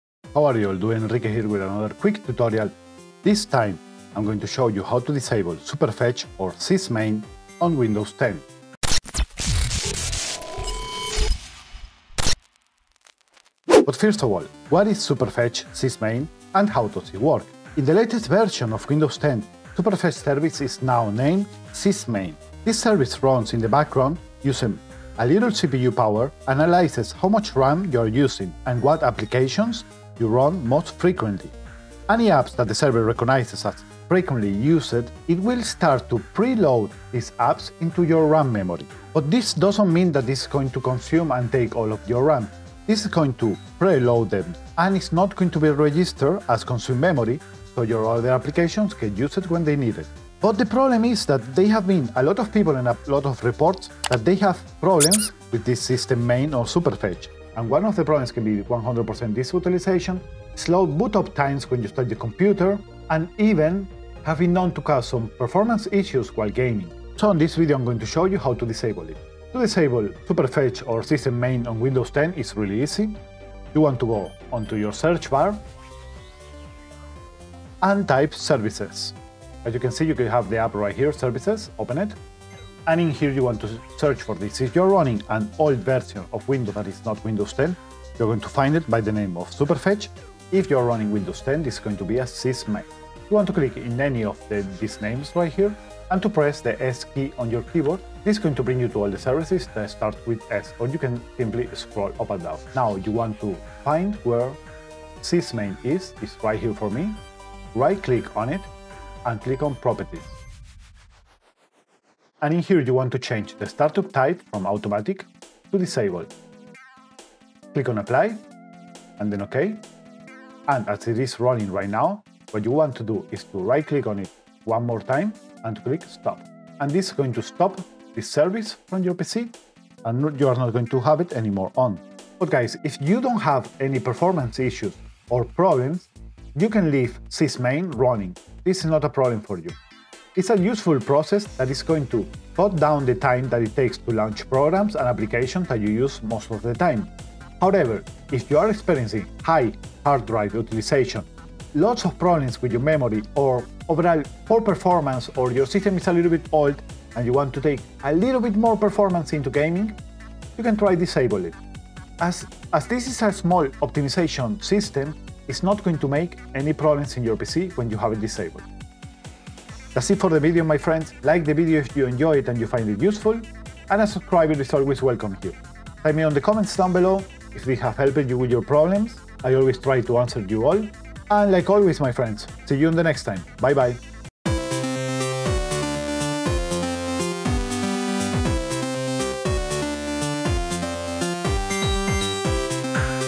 How to FIX 100% Disk utilization | Sysmain Superfetch
Fix 100% Disk Utilization on Windows 10, Here you are going to see how to disable Sysmain or Superfetch to fix the issues of 100% Disk utilization and also if yo have the problem of 100% Cpu Utilization because of this Windows Service.

Note:
On an windows update the "Superfetch" service was renamed "Sysmain", it's the same service however, If you can't find Sysmain, search for superfetch, it depends if you updated your system or not.

TimeStamps:
0:00 Intro
0:14 What is Sysmain or Superfetch
1:10 How to Disable Sysmain - Superfetch
2:12 Outro and Sugesstions

Designed by starline / Freepik